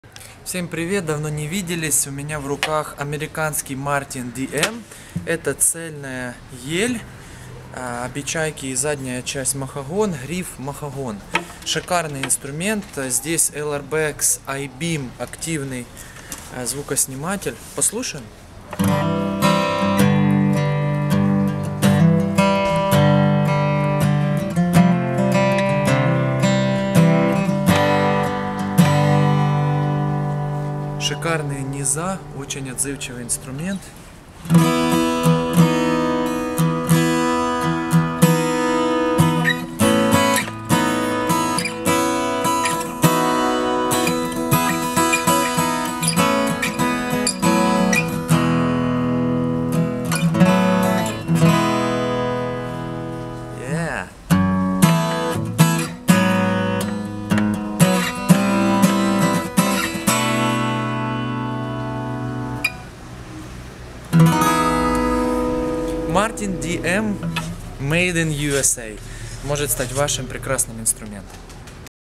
Martin DM Made in USA acoustic guitar by Rodion Avtushenko
For sale is great acoustic guitar
Solid Spruce top
Mahogany back and sides
Mahogany neck
LR Baggs I-beam active pickup